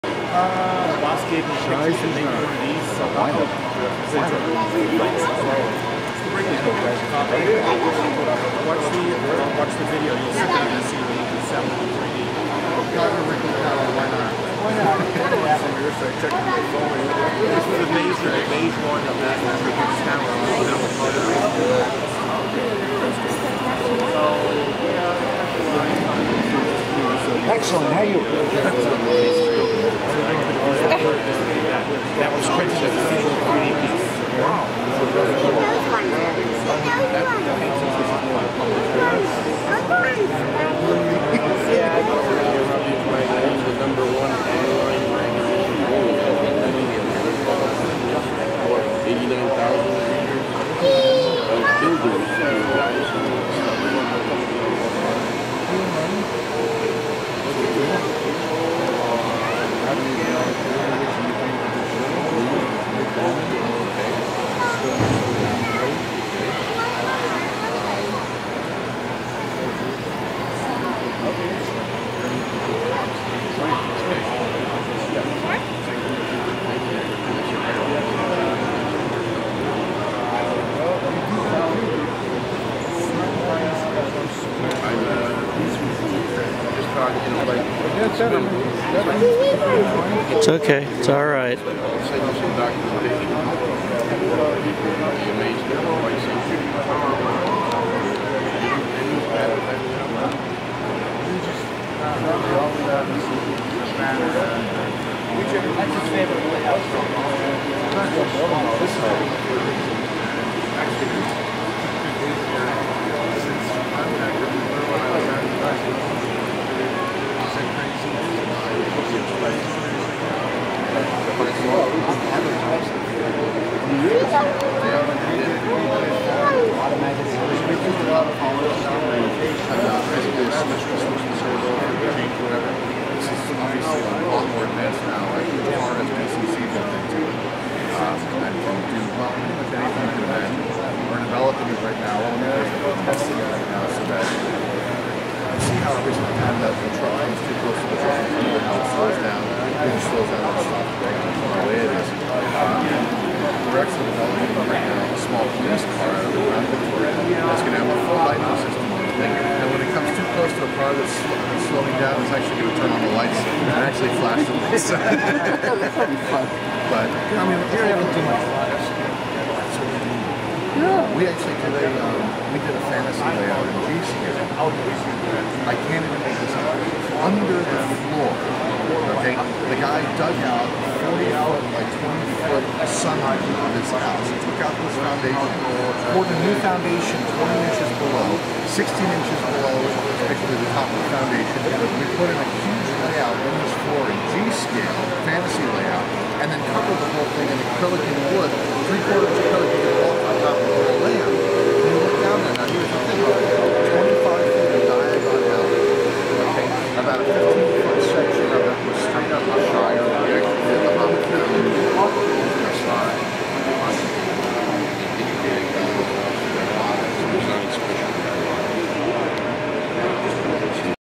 NTS 2017 - Smartt layout builder display
This is the display layout that Smartt brought to the 2017 NTS. It includes independently operating vehicles (they have IR sensors and emitters in them) and 3D printed structures. The crazy structure with the LED lighting is a 3D printed model of a proposed multi-hundred meter tall tower in Serbia. All the benchwork is 3/4" cabinet grade plywood and is designed to be interlocking.